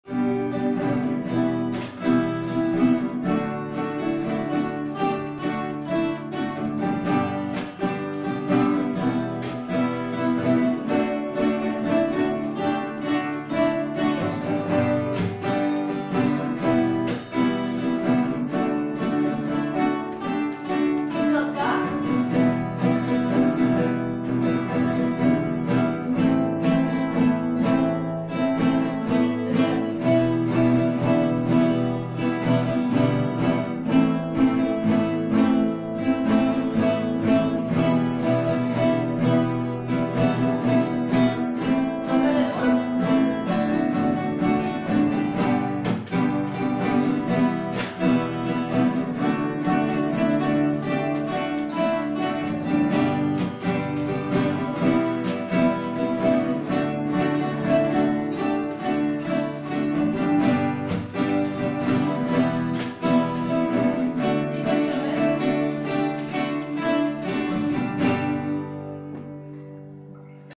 Zajęcia z gitary w GCK i KF
Intensywnie ćwiczy również klasa gitarowa